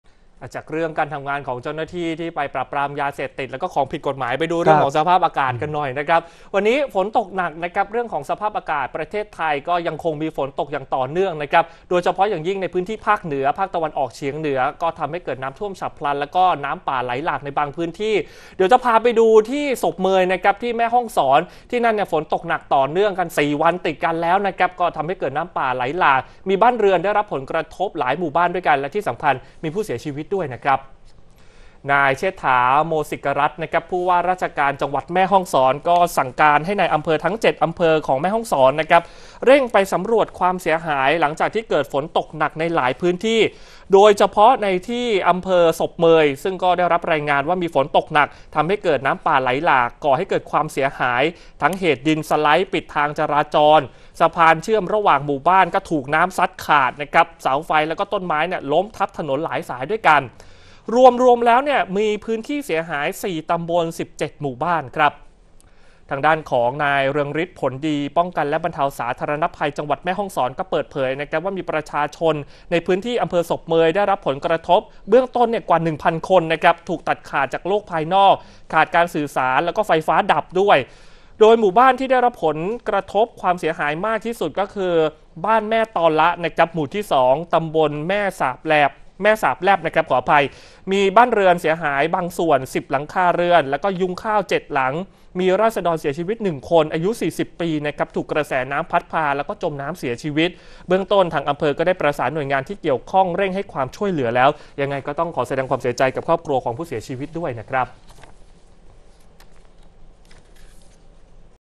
จ.แม่ฮ่องสอน เกิดน้ำป่าหลายหมู่บ้าน มีผู้เสียชีวิต 1 คน ข่าวค่ำ วันที่ 3 สิงหาคม 2566
เรื่องของสภาพอากาศ ที่ประเทศไทยยังคงมีฝนตกต่อเนื่อง โดยเฉพาะพื้นที่ภาคเหนือ ภาคตะวันออกเฉียงเหนือ ก็ทำให้เกิดน้ำท่วมฉับพลันและน้ำป่าในบางพื้นที่   โดยที่อำเภอสบเมย จังหวัดแม่ฮ่องสอน  มีฝนตกหนักต่อเนื่องกัน 4 วันทำให้เกิดน้ำป่าหลาก มีบ้านเรือนได้รับผลกระทบหลายหมู่บ้านและมีผู้เสียชีวิตด้วย